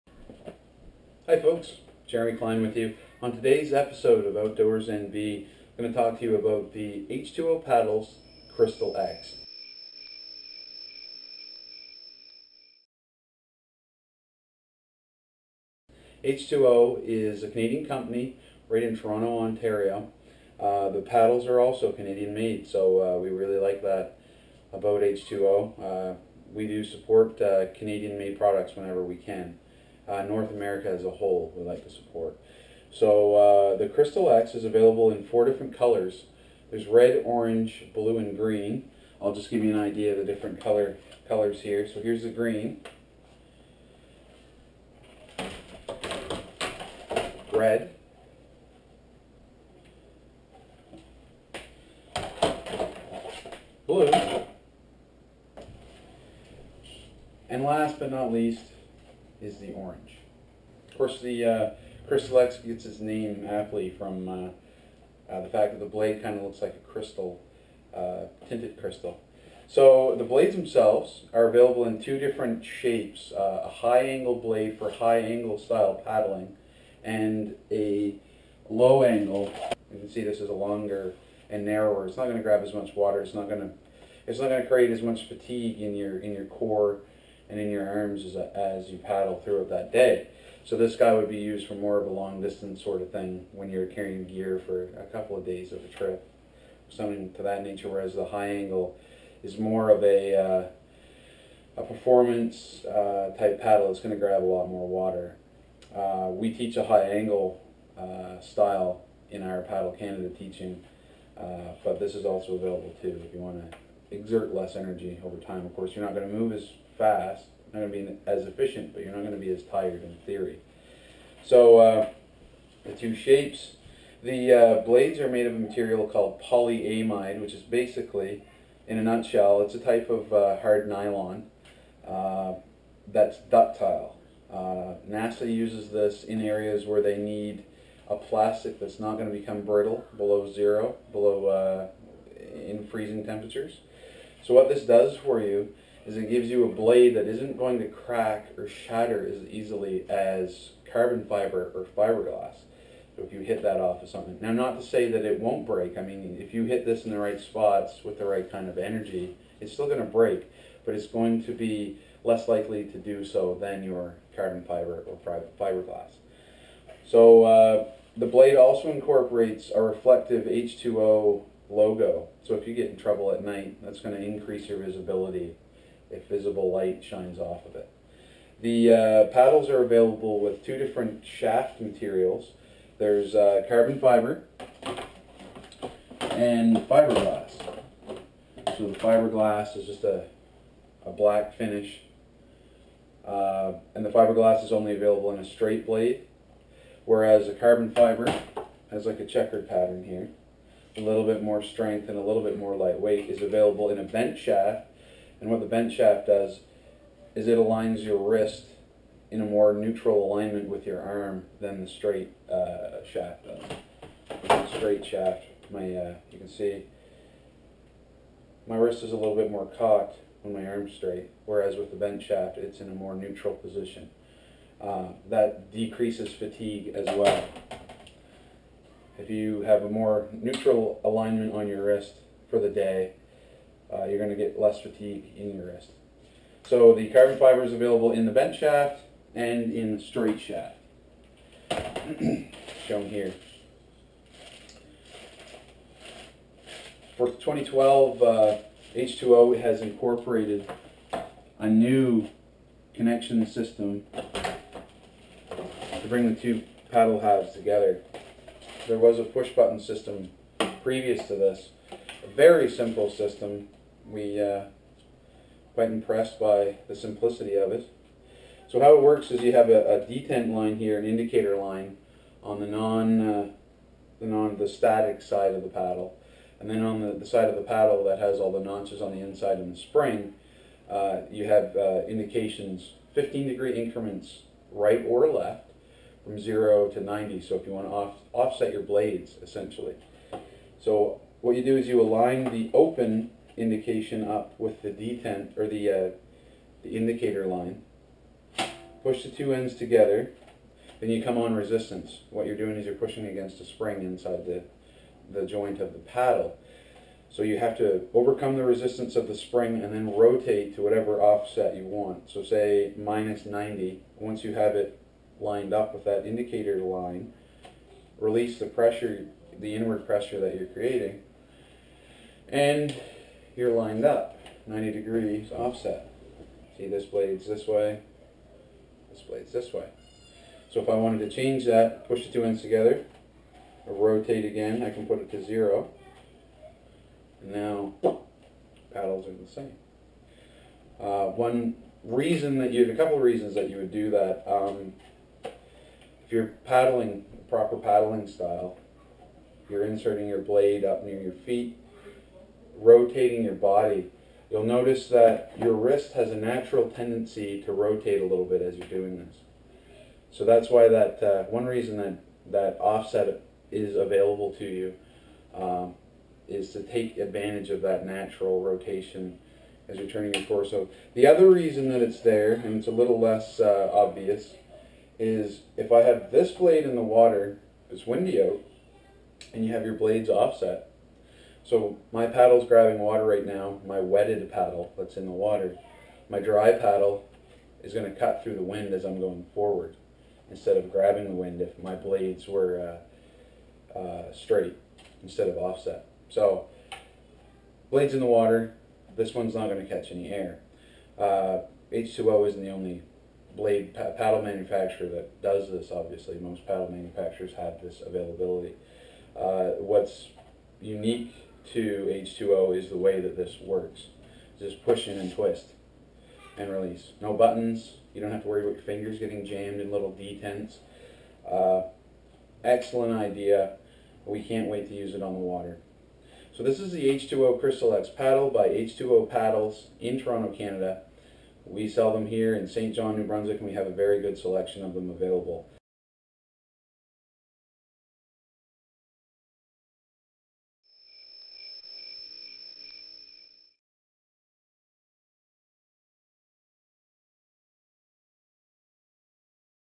Crystal X Overview
OutdoorsNB Inc's VP of Business Development overviews the H2O Paddles Crystal X paddle.